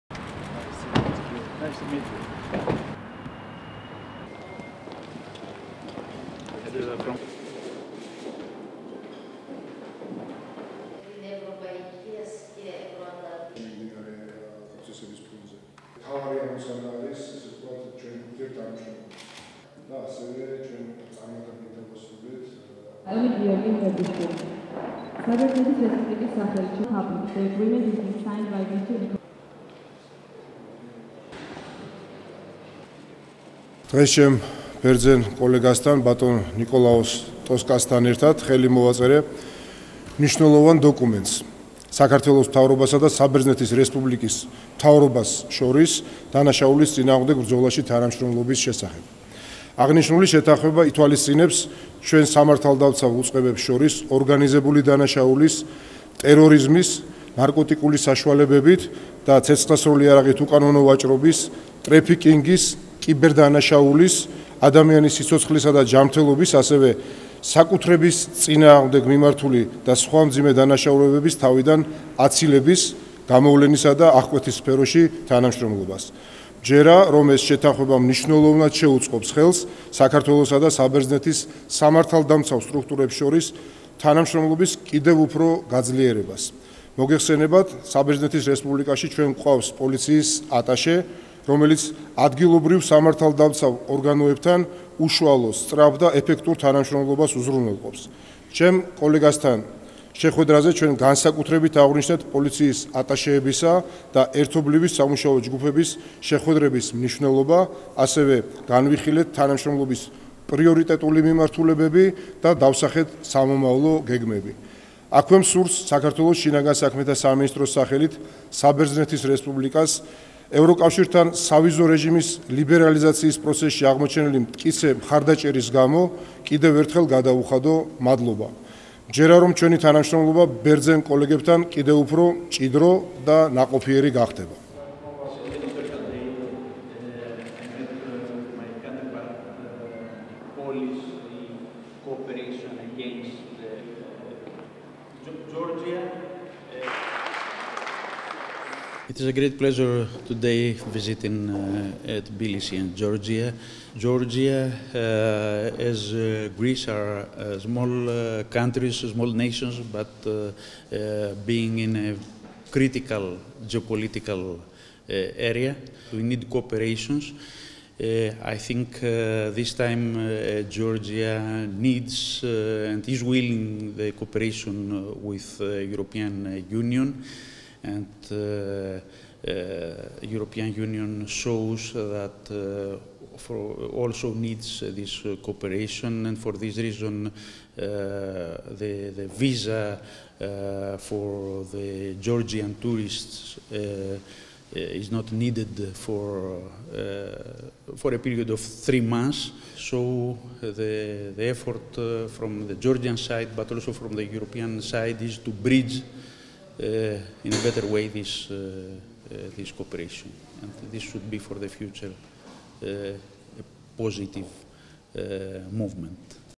საქართველოს საბერძნეთის შს მინისტრი პირველად ეწვია
საქართველოსა და საბერძნეთის შს მინისტრებმა დანაშაულის წინააღმდეგ ბრძოლაში თანამშრომლობის შესახებ შეთანხმებას ხელი მოაწერეს

შინაგან საქმეთა სამინისტროს, სამუშაო ვიზიტით, საბერძნეთის რესპუბლიკის შს მინიტრი ნიკოლაოს ტოსკასი დელეგაციასთან ერთად ეწვია. ბერძენ კოლეგას საქართველოს შს მინისტრი გიორგი მღებრიშვილი შეხვდა.

გიორგი მღებრიშვილმა და ნიკოლაოს ტოსკასმა ხელი მოაწერეს „საქართველოს მთავრობასა და საბერძნეთის რესპუბლიკის მთავრობას შორის დანაშაულის წინააღმდეგ ბრძოლაში თანამშრომლობის შესახებ“ შეთანხმებას.

საერთაშორისო ხელშეკრულება ითვალისწინებს მხარეებს შორის ორგანიზებული დანაშაულის, ტერორიზმის, ნარკოტიკული საშუალებებითა და ცეცხლსასროლი იარაღით უკანონო ვაჭრობის, ტრეფიკინგის, ადამიანის სიცოცხლისა და ჯანმრთელობის, ასევე, საკუთრების წინააღმდეგ მიმართული და სხვა მძიმე დანაშაულების თავიდან აცილების, გამოვლენისა და აღკვეთის სფეროში თანამშრომლობას.

როგორც მხარეებმა ხელმოწერის ცერემონიის შემდეგ აღნიშნეს, დანაშაულის წინააღმდეგ ბრძოლაში თანამშრომლობის შესახებ შეთანხმება მნიშვნელოვნად შეუწყობს ხელს საქართველოსა და საბერძნეთის რესპუბლიკის სამართალდამცავ ორგანოებს შორის ურთიერთობის კიდევ უფრო გაღრმავებას.

პირისპირ შეხვედრაზე გიორგი მღებრიშვილმა და ნიკოლაოს ტოსკასმა ხაზი გაუსვეს პოლიციის ატაშეებისა და ერთობლივი სამუშაო ჯგუფების მნიშვნელობას, თანამშრომლობის პრიორიტეტული მიმართულებები განიხილეს და სამომავლო გეგმები დასახეს.

საქართველოს შს მინისტრმა ბერძენ კოლეგას ევროკავშირთან სავიზო რეჟიმის ლიბერალიზაციის პროცესში საბერძნეთის რესპუბლიკის მიერ გამოხატული მტკიცე მხარდაჭერისთვის მადლობა გადაუხადა.